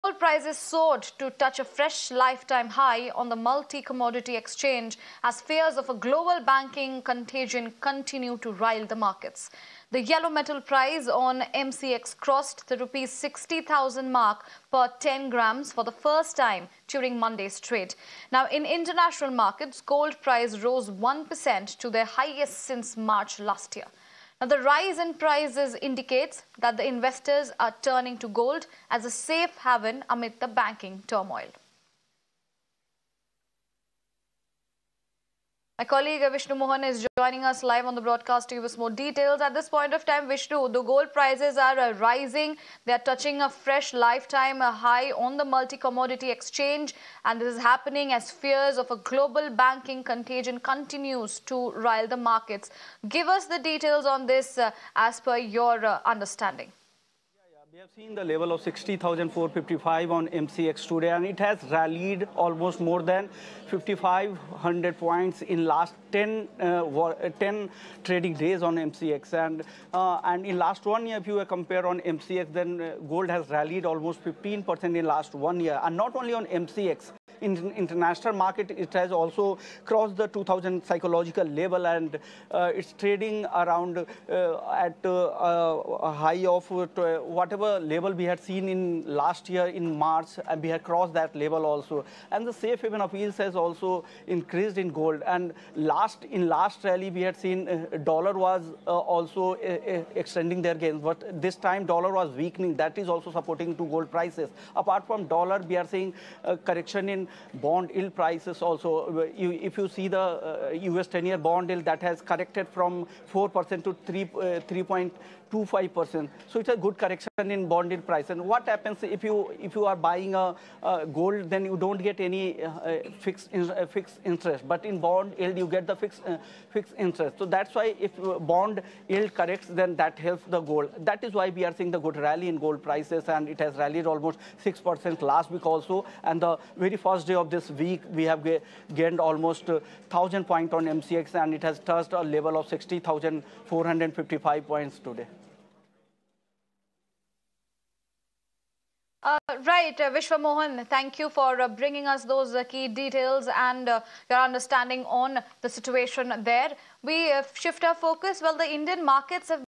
Gold Prices Hit Fresh Lifetime High On Multi Commodity Exchange; Crosses Rs 60,000-Mark Per 10 Grams
As per the latest news on Mirror Now, gold prices soared to touch a fresh lifetime high on multi-commodity exchange as fierce global banking contagion continues to rile the markets. The yellow metal price on the MCX crossed the Rs.60,000 mark per 10 gms for the first time during Monday's trade. In the international market gold price rose one percent to their highest since March last year. A rise in price indicates that investors are turning to gold as a safe haven amid the banking turmoil.